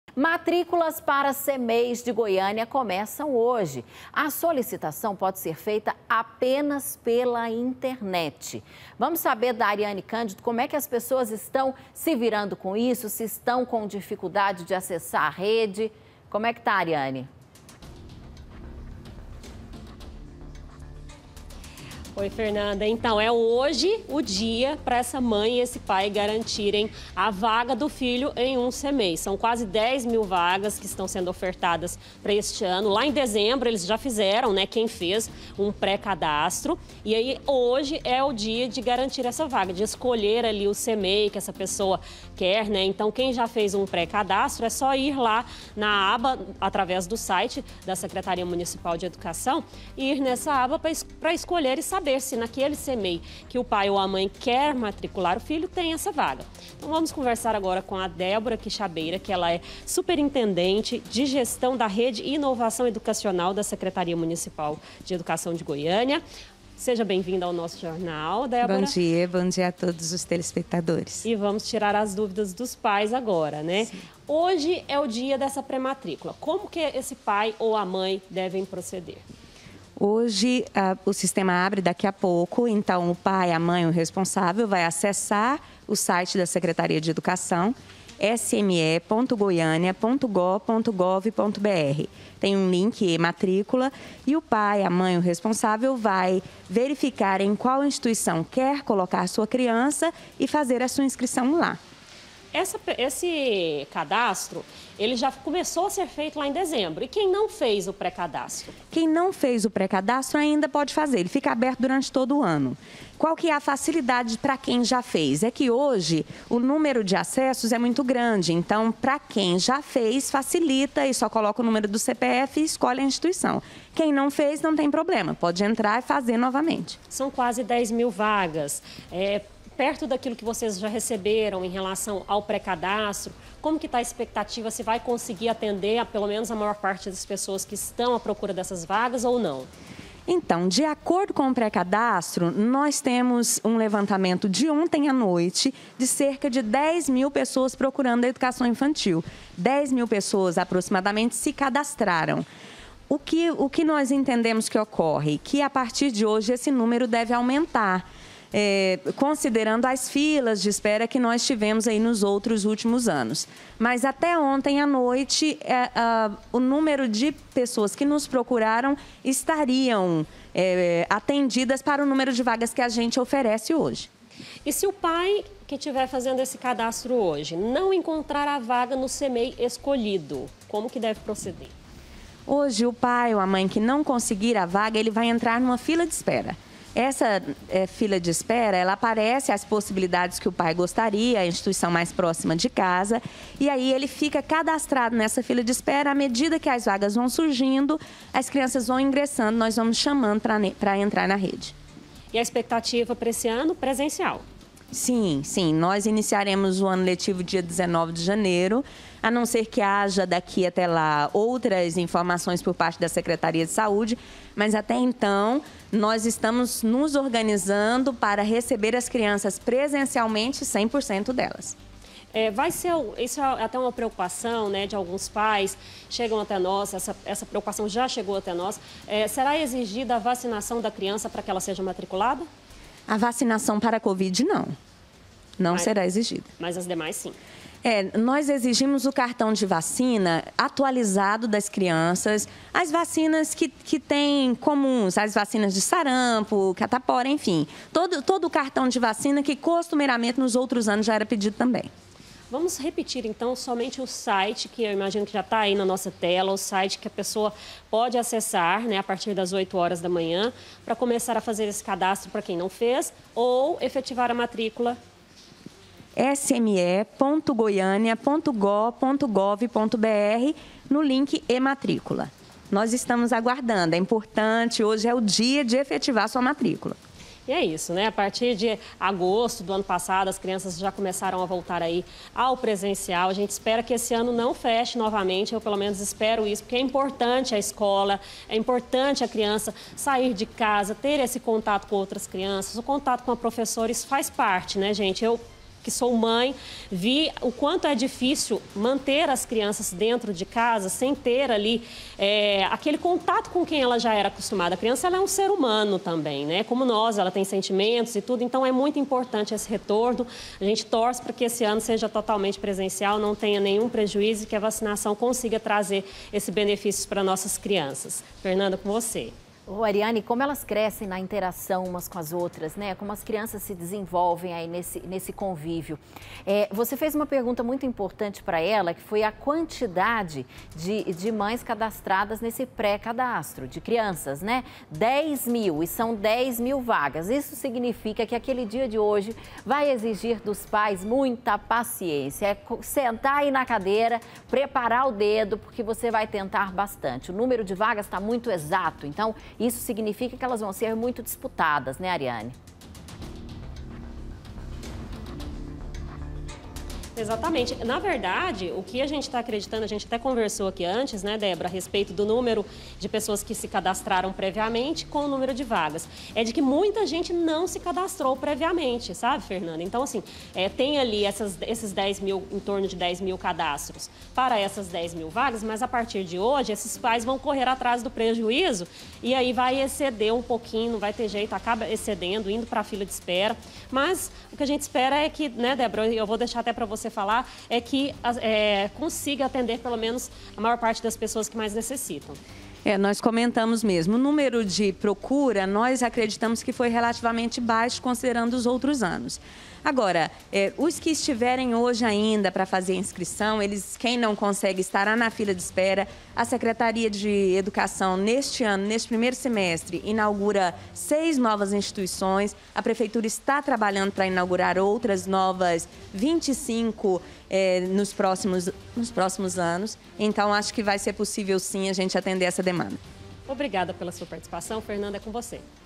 CMEIS DE GOIÂNIA: MATRÍCULAS COMEÇAM HOJE PELA INTERNET
MATRÍCULAS PARA CMEIS DE GOIÂNIA COMEÇAM HOJE. A SOLICITAÇÃO PODE DER FEITA APENAS PELA INTERNET. ARIANNE CÂNDIDO ESSE SISTEMA NÃO DIFICULTA A VIDA DE PESSOAS QUE NÃO TÊM ACESSO À REDE?